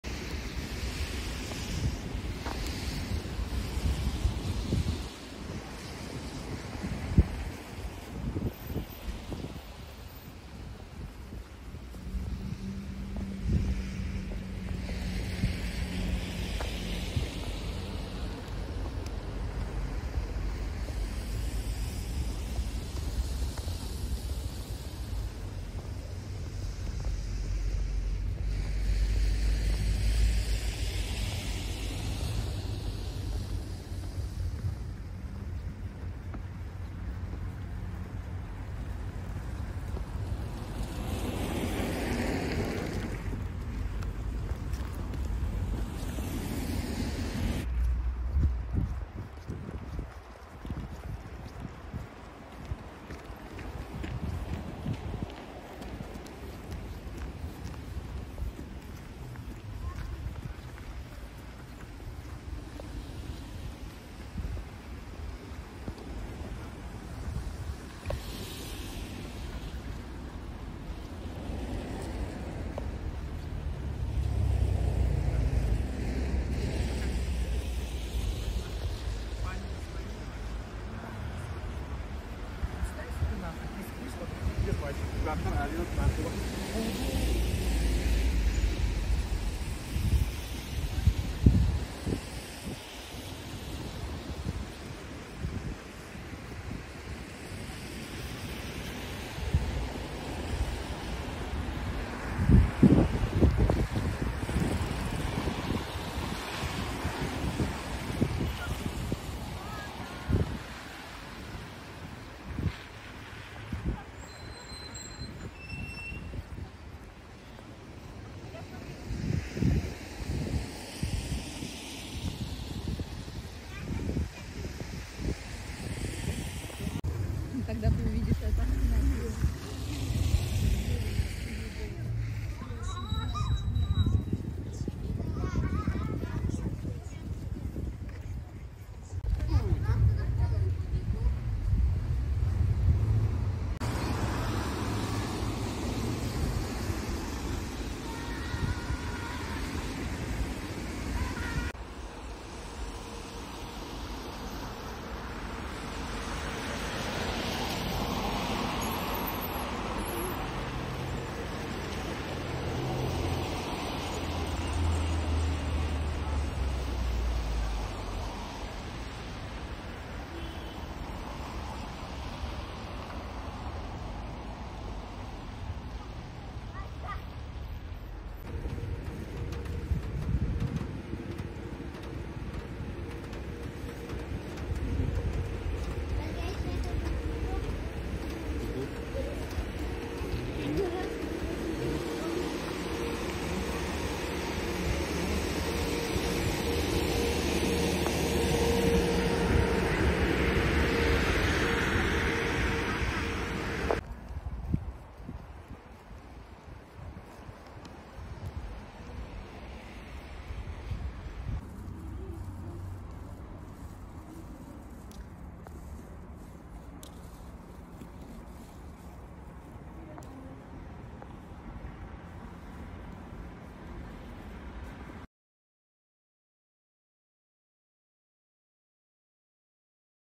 Petrovsky Street, where Jews used to live
In Yakutsk, Jews lived compactly in the area of ​​the current Petrovsky Street - a synagogue was built there, a Russian-Jewish school worked, a Jewish club acted as a pledge